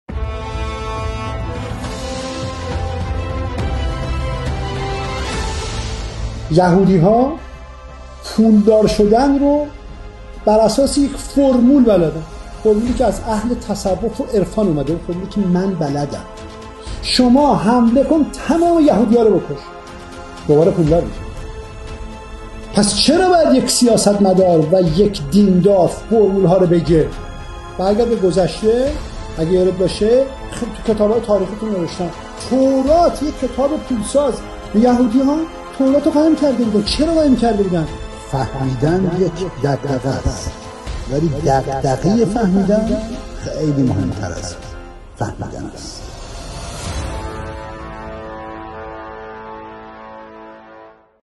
فرمول پولدار شدن
🔴 تورات و ثروت یهودیان

رازی که اگر بدانی کشورت نجات می‌یابد!

لیدر ری استارت، سید محمد حسینی
RESTART Leader, Seyed Mohammad Hosseini